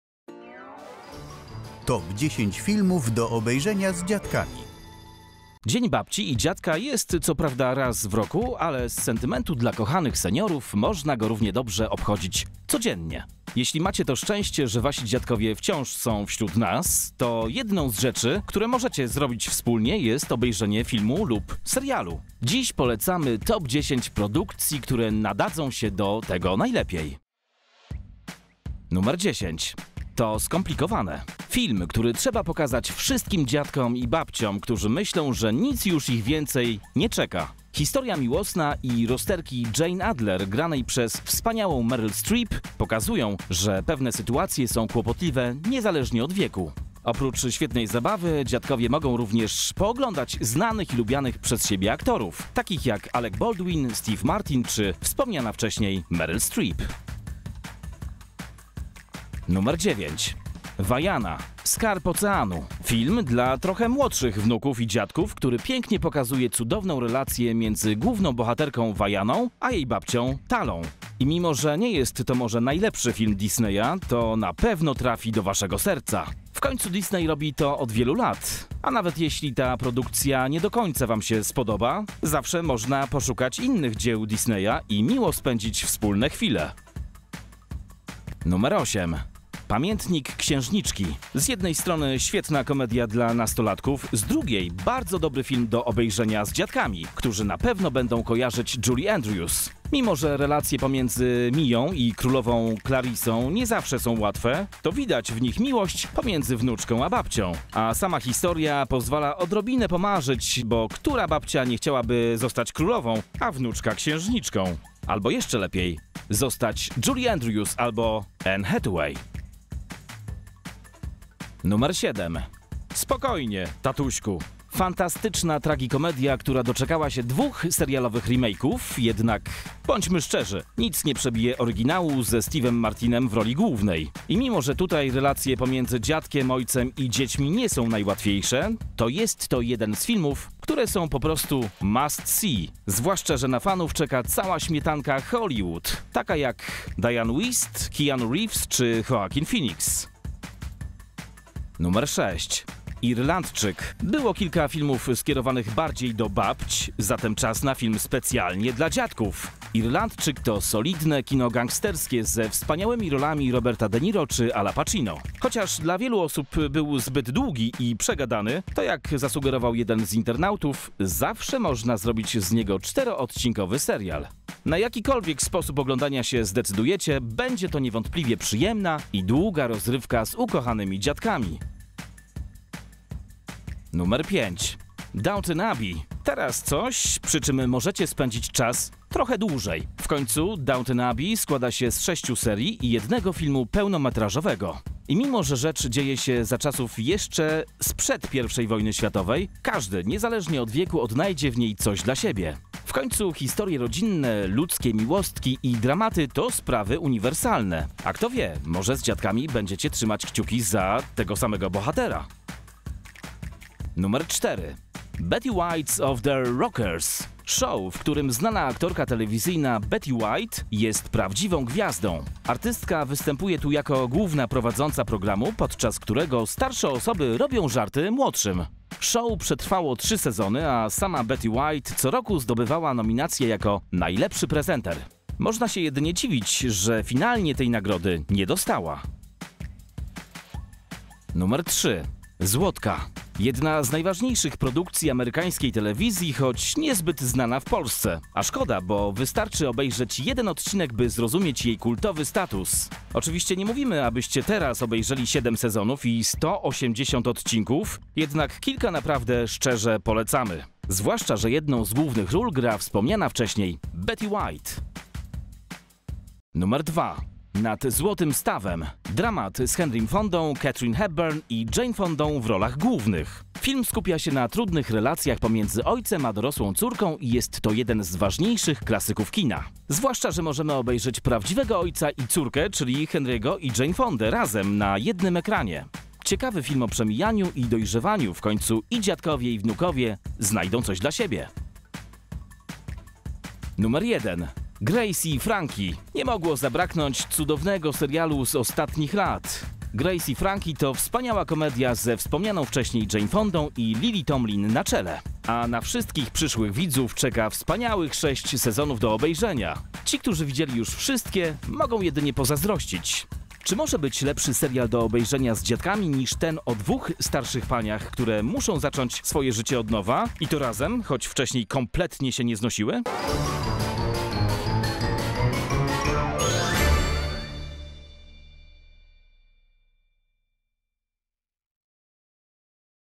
TOP 10 - FILMY DO OBEJRZENIA Z DZIADKAMI
Dzień babci i dziadka jest co prawda raz do roku, ale z sentymentu do kochanych seniorów można go równie dobrze obchodzić... codziennie! Jeśli macie to szczęście, że Wasi dziadkowie wciąż są wśród nas, to jedną z rzeczy, które możecie zrobić wspólnie, jest obejrzenie filmu lub serialu. Dziś polecamy naszą TOP 10 produkcji, które świetnie się do tego nadadzą. :)